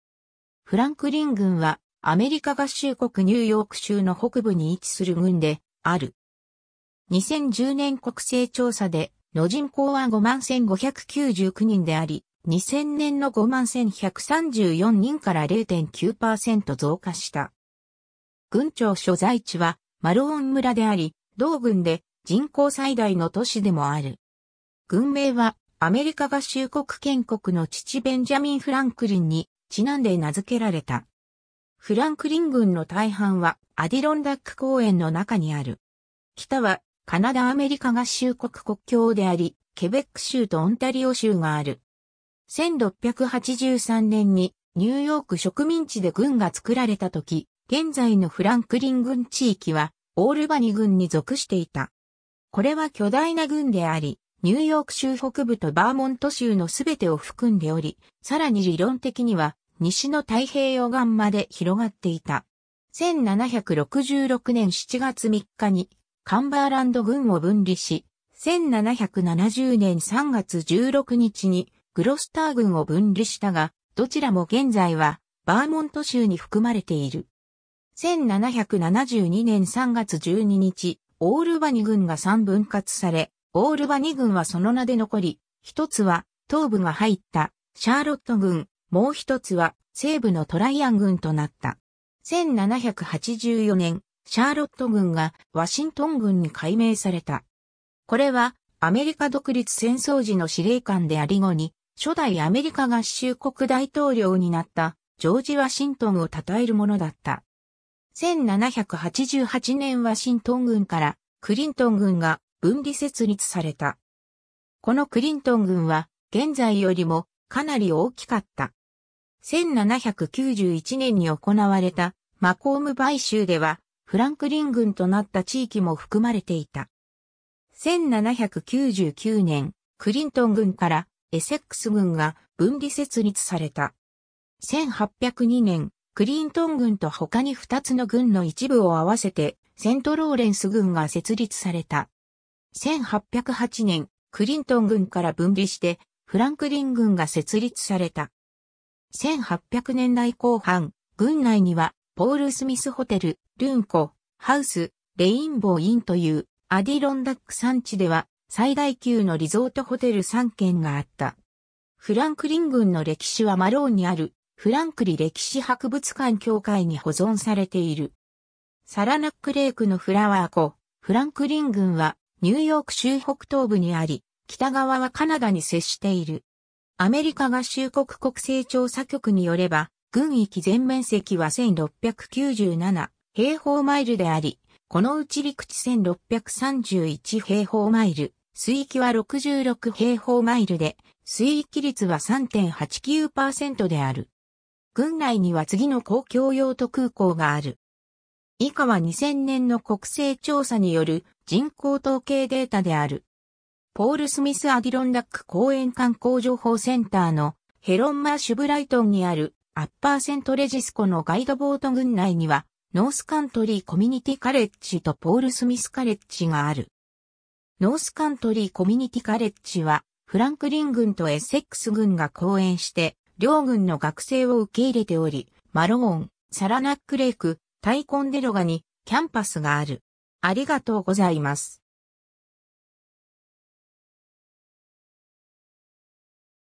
フランクリン郡 (ニューヨーク州)
ベンジャミン・フランクリン
フランクリン郡（英: Franklin County）は、アメリカ合衆国ニューヨーク州の北部に位置する郡である。
2010年国勢調査での人口は51,599人であり、2000年の51,134 人から0.9%増加した。
郡庁所在地はマローン村（人口5,911人）であり、同郡で人口最大の都市でもある。
郡名はアメリカ合衆国建国の父ベンジャミン・フランクリンに因んで名付けられた。
 フランクリン郡の大半はアディロンダック公園の中にある。
北はカナダ＝アメリカ合衆国国境であり、ケベック州とオンタリオ州がある。
 1683年にニューヨーク植民地で郡が作られたとき、現在のフランクリン郡地域はオールバニ郡に属していた。
これは巨大な郡であり、ニューヨーク州北部とバーモント州の全てを含んでおり、さらに理論的には西の太平洋岸まで広がっていた。
1766年7月3日に、カンバーランド郡を分離し、1770年3月16日にグロスター郡を分離したが、どちらも現在はバーモント州に含まれている。
1772年3月12日、オールバニ郡が3分割され、オールバニ郡はその名で残り、1つは東部が入ったシャーロット郡、もう1つは西部のトライアン郡となった。
 1784年、シャーロット郡がワシントン郡に改名された。
これはアメリカ独立戦争時の司令官であり後に初代アメリカ合衆国大統領になったジョージ・ワシントンを称えるものだった。
 1788年ワシントン郡からクリントン郡が分離設立された。
このクリントン郡は現在よりもかなり大きかった。
 1791年に行われたマコーム買収では、フランクリン郡となった地域も含まれていた。
 1799年、クリントン郡からエセックス郡が分離設立された。
1802年、クリントン郡と他に2つの郡の一部を併せて、セントローレンス郡が設立された。
 1808年、クリントン郡から分離してフランクリン郡が設立された。
 1800年代後半、郡内にはポール・スミス・ホテル、ルーン湖ハウス、レインボウ・インという、アディロンダック山地では最大級のリゾートホテル3軒があった。
 フランクリン郡の歴史はマローンにあるフランクリ歴史博物館協会に保存されている。
 サラナックレイクのフラワー湖 フランクリン郡はニューヨーク州北東部にあり、北側はカナダに接している。
 アメリカ合衆国国勢調査局に拠れば、郡域全面積は1,697平方マイル (4,395.2 km2)であり、このうち陸地1,631平方マイル (4,224.3 km2)、水域は66平方マイル (170.9 km2)で水域率は3.89%である。
 郡内には次の公共用途空港がある。
 ポール・スミス・アディロンダック公園観光情報センターのヘロン・マーシュ ブライトンにあるアッパー・セントレジス湖のガイドボート 郡内にはノースカントリー・コミュニティカレッジとポール・スミス・カレッジがある。
ノースカントリー・コミュニティカレッジはフランクリン郡とエセックス郡が後援して両郡の学生を受け入れており、マローン、サラナックレイク、タイコンデロガにキャンパスがある。